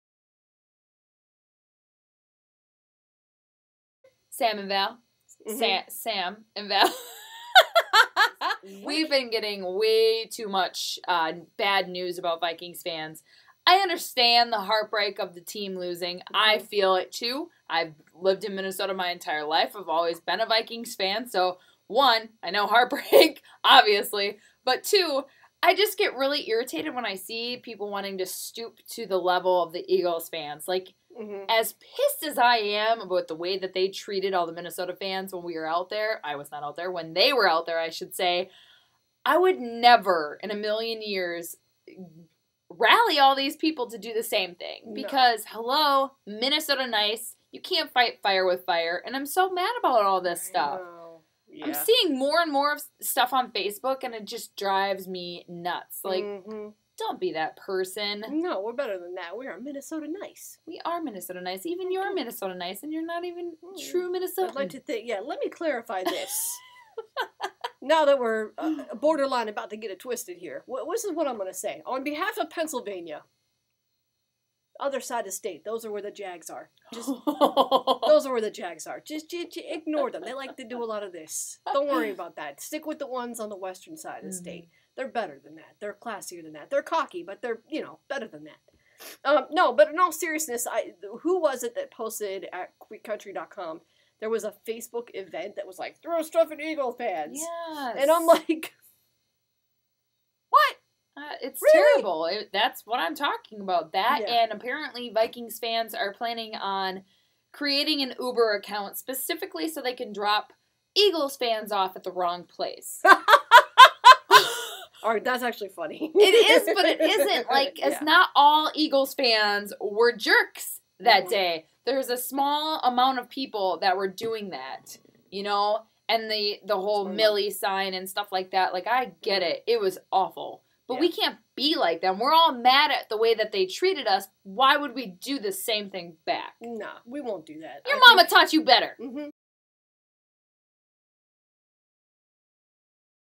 A Message to Minnesota Vikings Fans
On today's episode of Girl Talk with Samm and Val, we have a message for Vikings fans. We are not impressed with Minnesota fans stooping to Philly's level. Just don't!